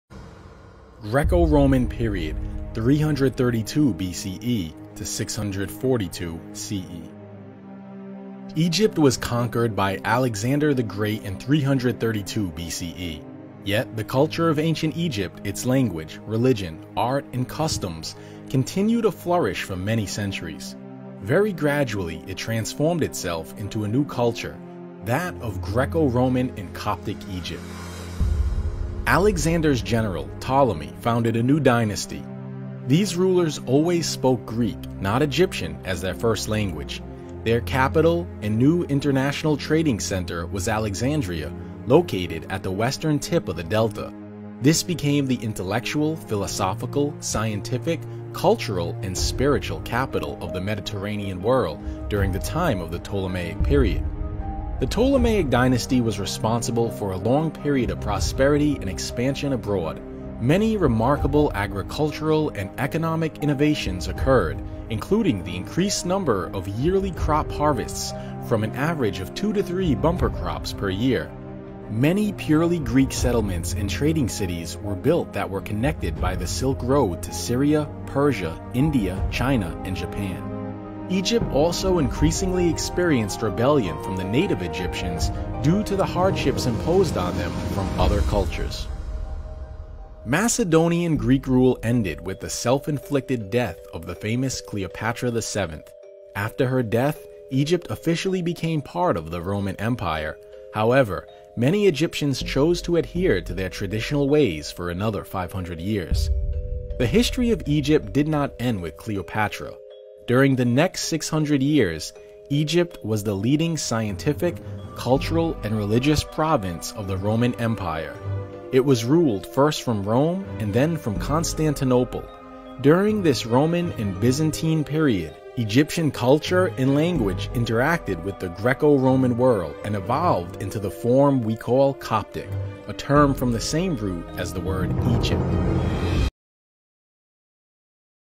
Egypt of the Greco Roman period
The Late Period of Ancient Egyptian history came to an end in 332 BC when Egypt was conquered by the Greeks. The Greeks formed their own dynasty called the Ptolemaic Dynasty that ruled for nearly 300 years until 30 BC. In 30 BC the Romans took control of Egypt. The Romans ruled for over 600 years until around 640 AD.